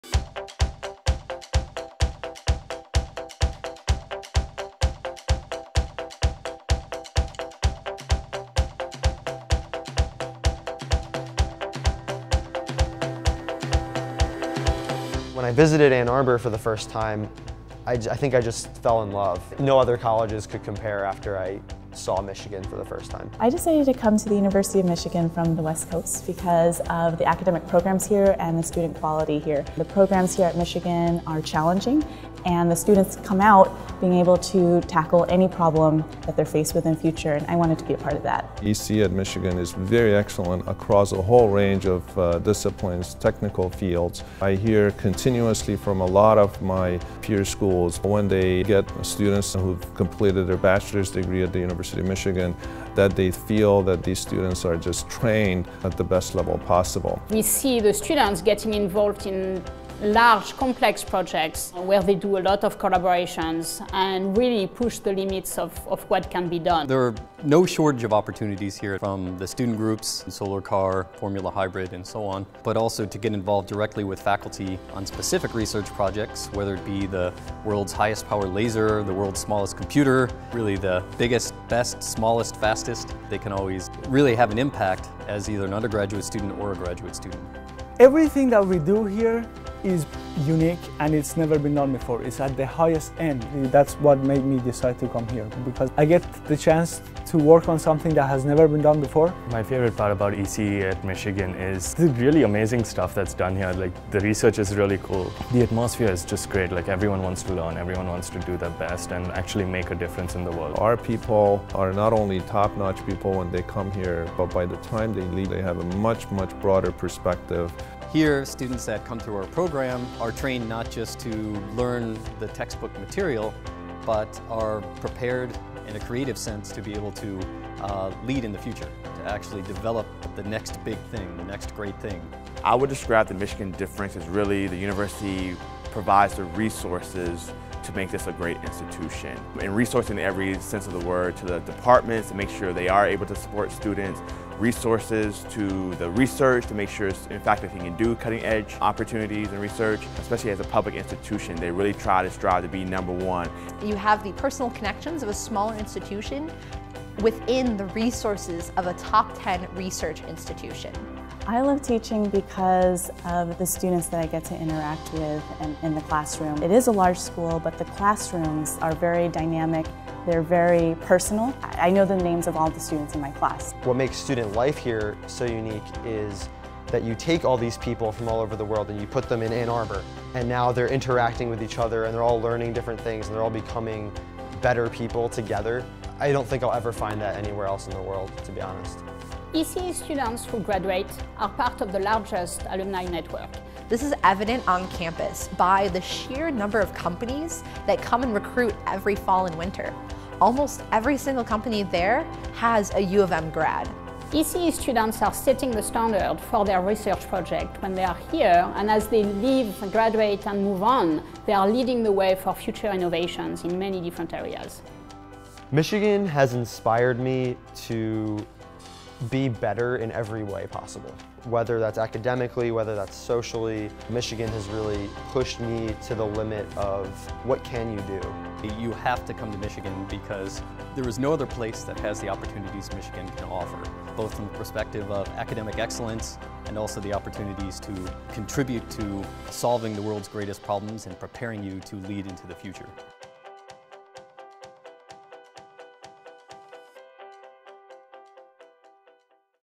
Electrical & Computer Engineering (ECE) at Michigan
Welcome to Electrical and Computer Engineering at Michigan!  We have 2 of the 100 top 10 ranked programs in the University of Michigan, thanks to our expertise in research, entrepreneurship, teaching, and overall excellence. We care about our students. Come visit!